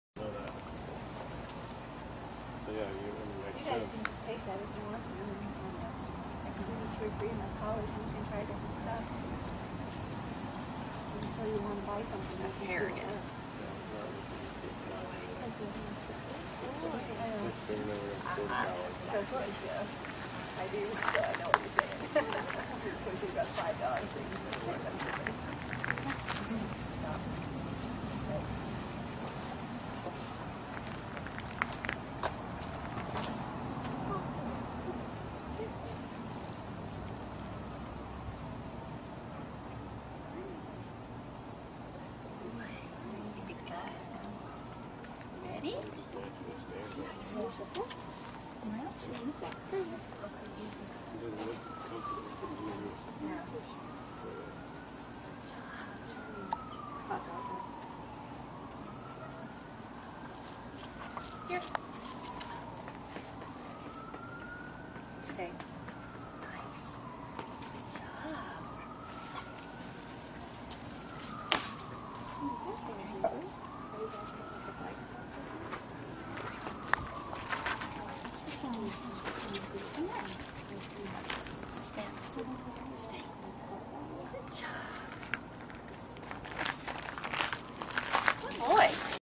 Jimmer's Handling Video
On May 26th, Jimmer went to his first handling class taught by Shawn Swanson of Maverick Dog Training in Glendale Arizona.  This was his first time being handled by Shawna.  They look great.  Jimmer is 5 months old.  Video taken by Maverick Dog Training